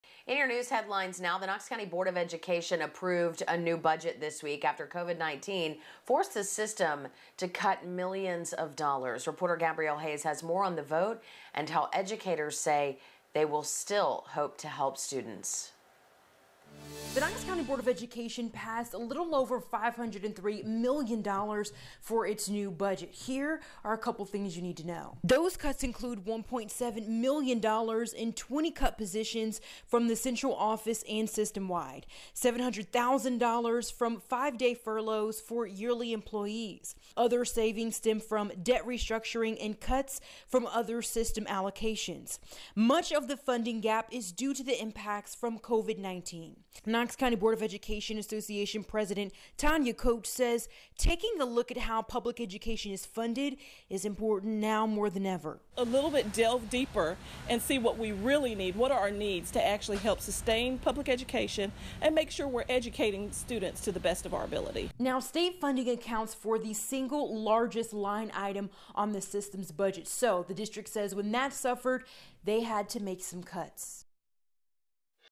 Knox County Board of Education cuts $4 million from budget
The Knox County Board of education approved a new budget this week after COVID-19 forced the system to cut millions.